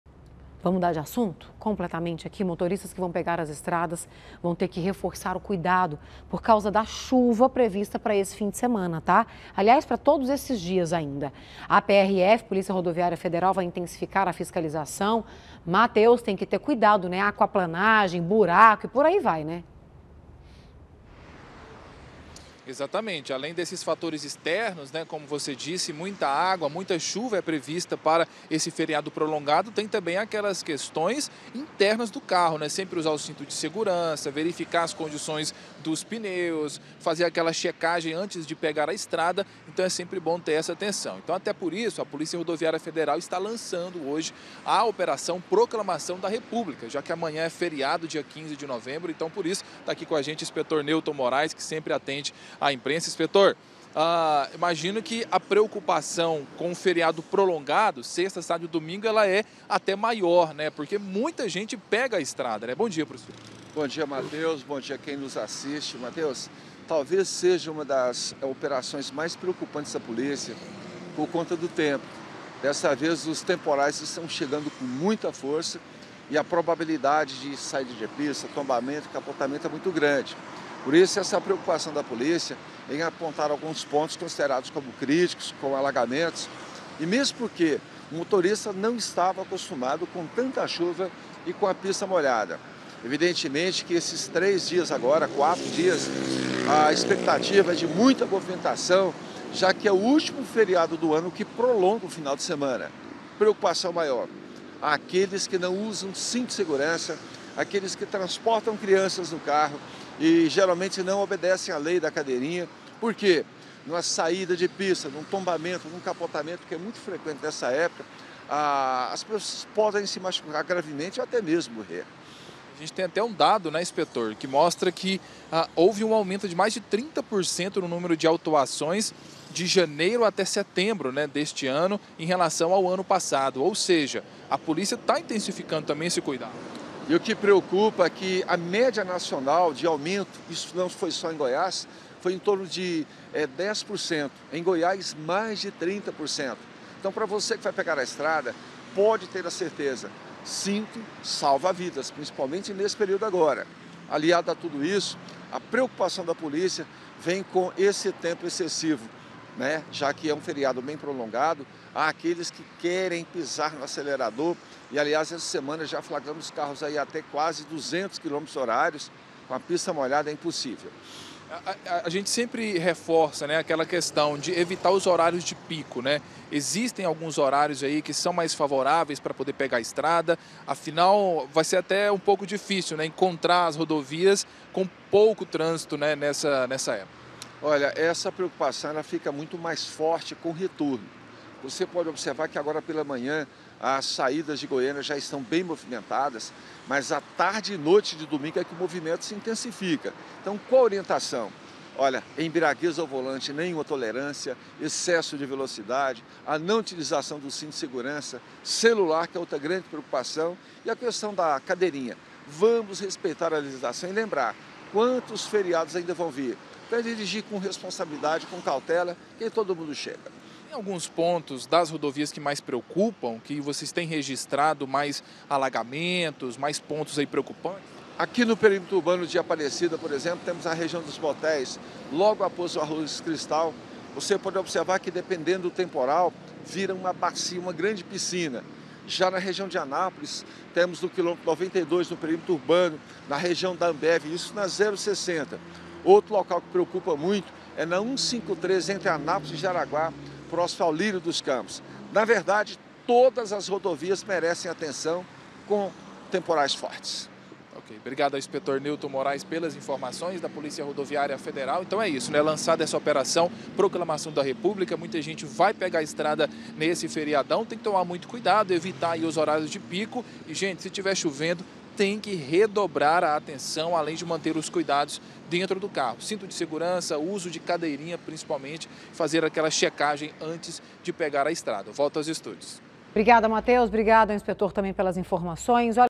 CHUVAS FORTES PODEM PROVOCAR ACIDENTES
MOTORISTAS QUE VÃO PEGAR AS ESTRADAS TERÃO QUE REFORÇAR O CUIDADO, POR CAUSA DAS CHUVAS PREVISTAS PRA ESSE FIM DE SEMANA. A PRF VAI INTENSIFICAR A FISCALIZAÇÃO.